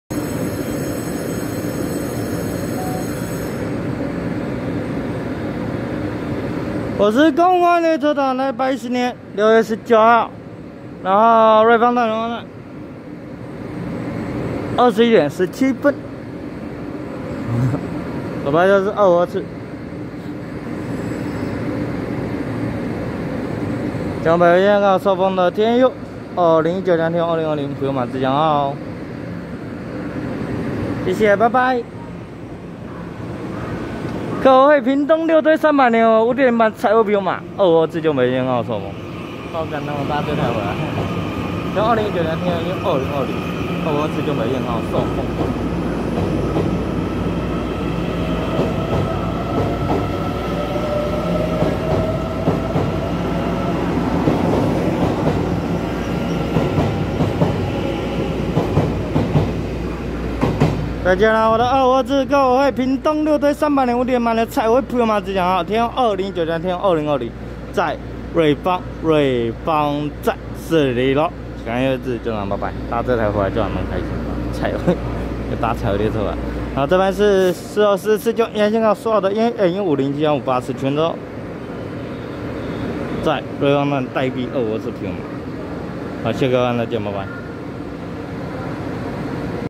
252次樹林發車經由北迴線開往壽豐的客委會屏東六堆300年5.0版TEMU2020+TEMU2019彩繪普悠瑪自強號，彩繪閃燈打招呼，和4244次開往蘇澳的EMU507+EMU584區間車
110/6/19瑞芳站21:17分拍攝252次樹林發車經由北迴線開往壽豐的客委會屏東六堆300年5.0版TEMU2020+TEMU2019彩繪普悠瑪自強號，彩繪閃燈打招呼，和4244次樹林發車經由宜蘭線開往蘇澳的EMU507+EMU584區間車